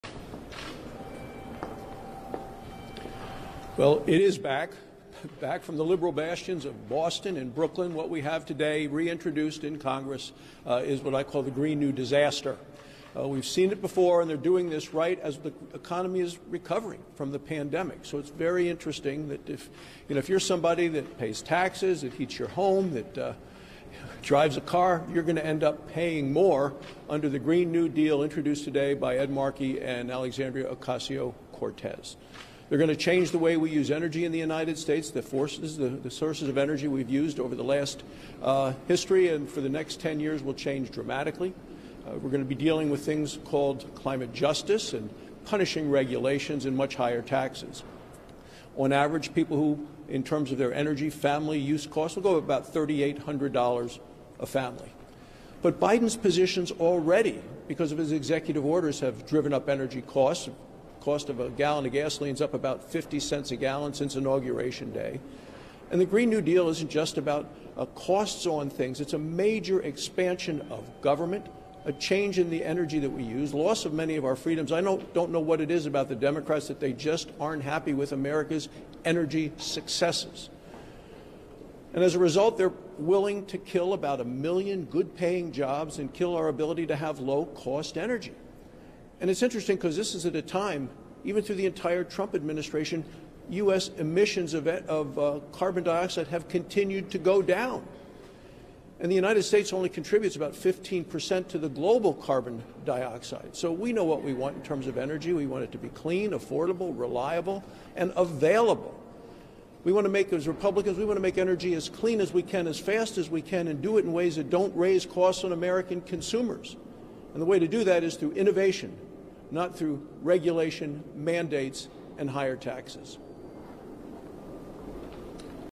"The Green New Disaster": GOP Senator Slams Climate Proposal From Markey and AOC
At a Senate GOP press conference yesterday, Sen. John Barrasso (R-WY) ripped the Green New Deal legislation reintroduced this week by Sen. Ed Markey (D-NY) and Rep. Alexandria Ocasio-Cortez (D-NY).